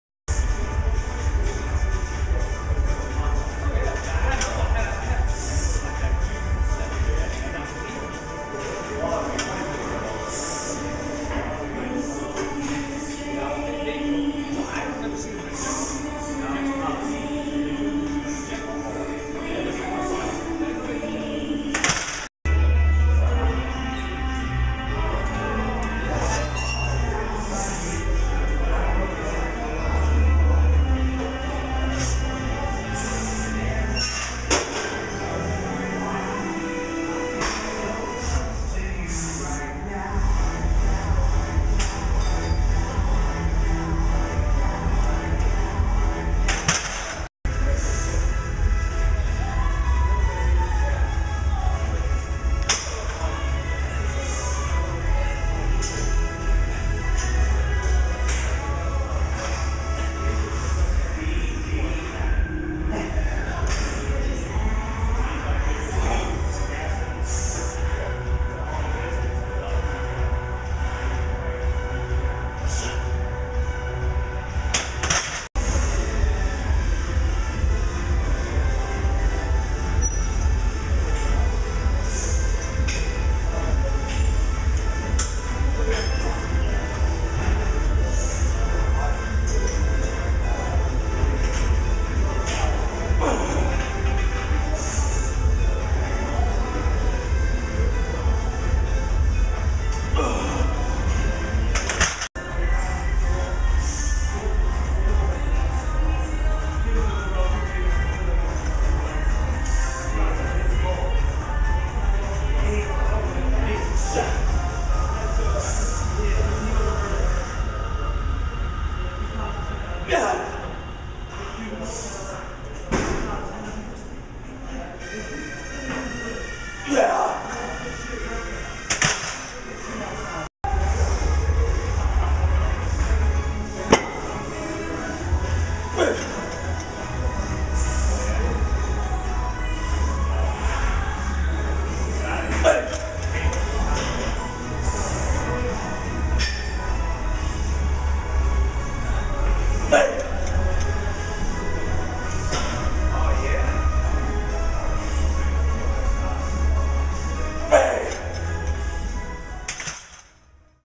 Squat - 330x4x6 (80%)
This was a lot harder than last week even though the video may not show it. I think it may deal with the fact that I'm not eating enough or drinking enough water as of late, especially before this training session. I've had a lot of things going on the past few days but now I am ready to focus again. Ready to kill bench on Thursday.